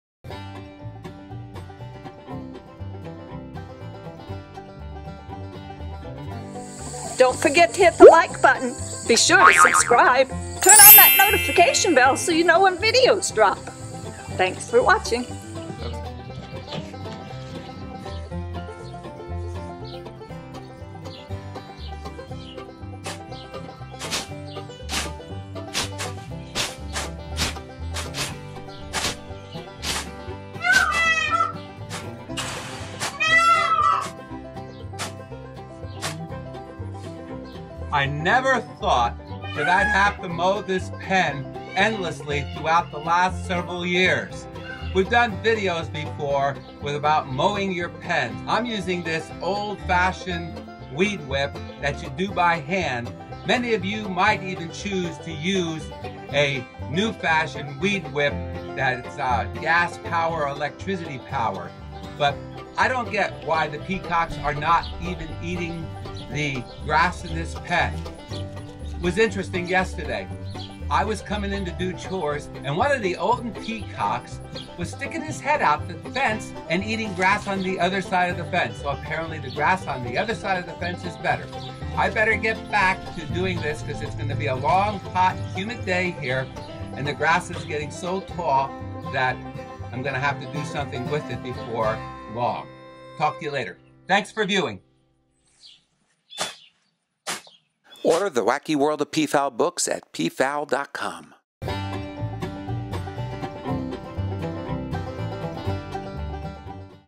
Weed Whipping Peacock Pen, Peacock Minute, peafowl.com
On the latest episode of a peacock minute, Mr. Peacock shows what he has to do in one of his peacocks pens, cut/weed whip grass in the peafowl pens.